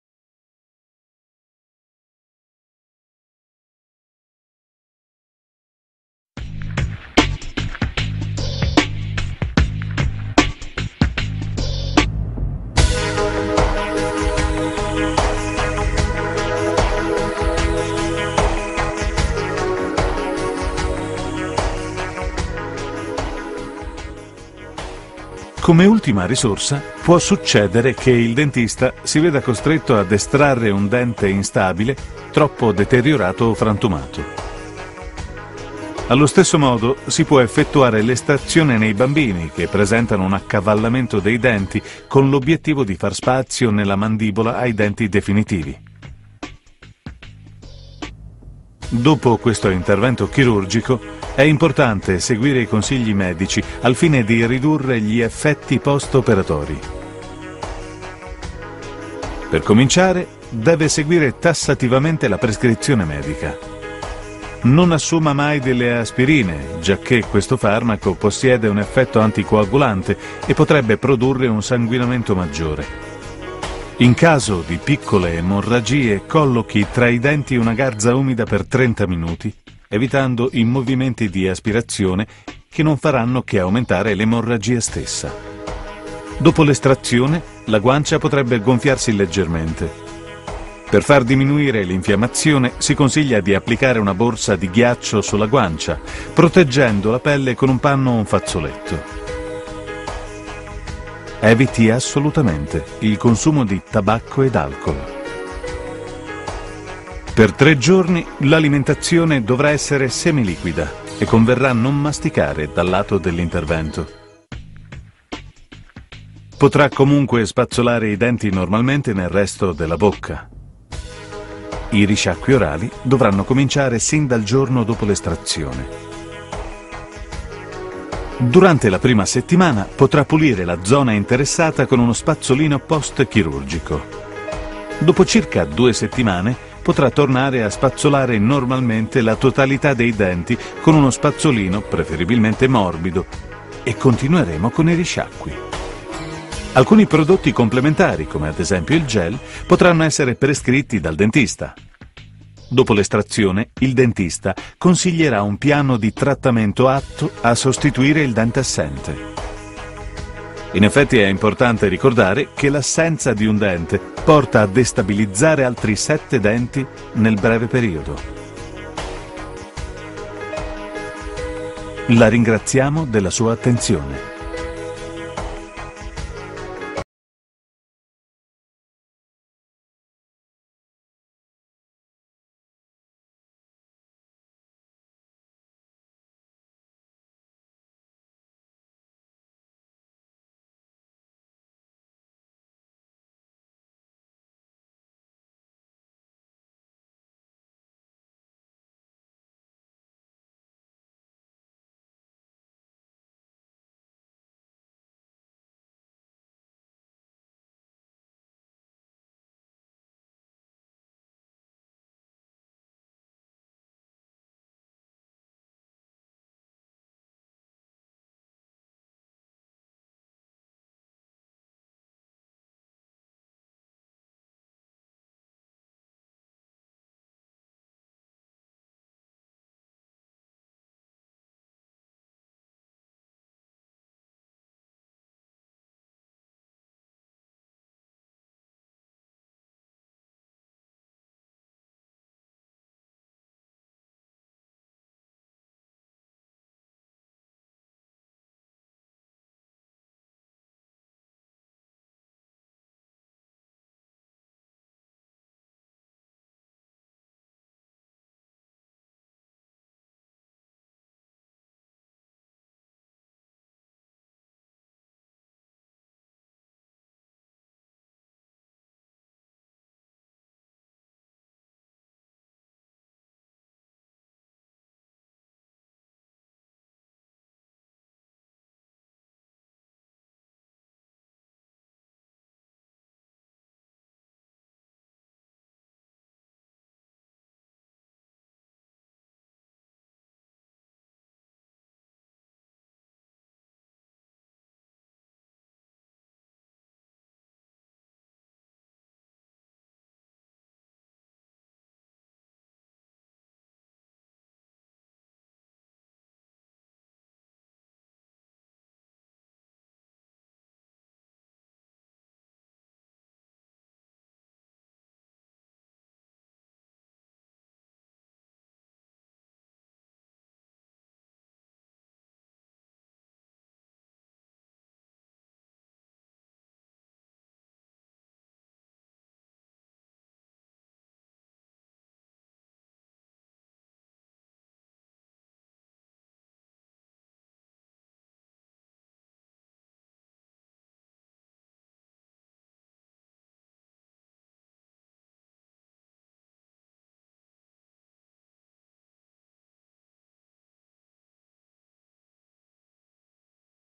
Estrazione del dente.
Estrazione del dente, consigli utili per il periodo post estrattivo di un dente normale o del dente di giudizio.